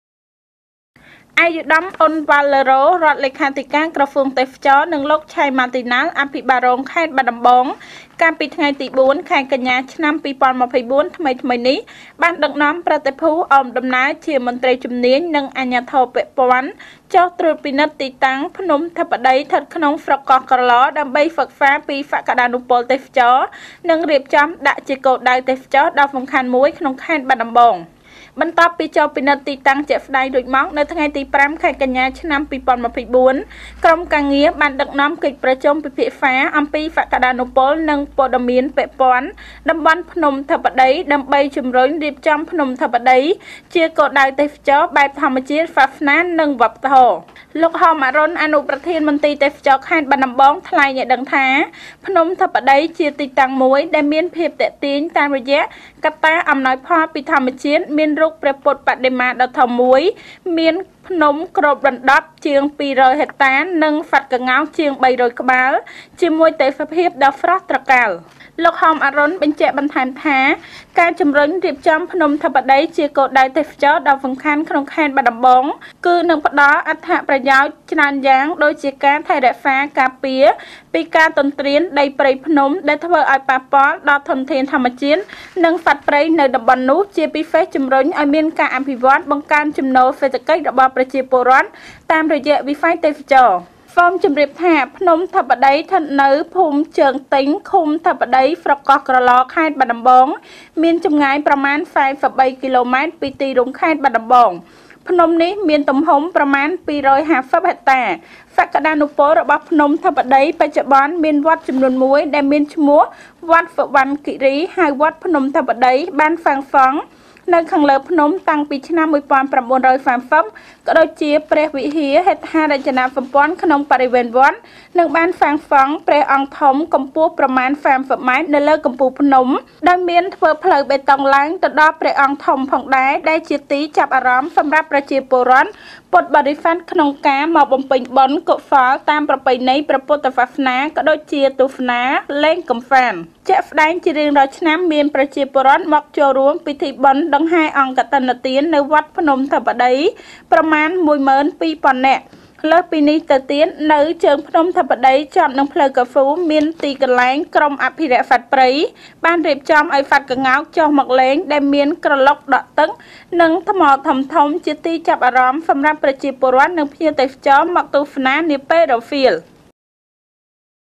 ក្រសួងទេសចរណ៍ ចុះសិក្សាសក្ដានុពលភ្នំធិបតី ដើម្បីរៀបចំដាក់ជាគោលដៅទេសចរណ៍ដ៏សំខាន់មួយក្នុងខេត្តបាត់ដំបង
ទទក: ក្រសួងទេសចរណ៍ ចុះសិក្សាសក្ដានុពលភ្នំធិបតី ដើម្បីរៀបចំដាក់ជាគោលដៅទេសចរណ៍ដ៏សំខាន់មួយក្នុងខេត្តបាត់ដំបង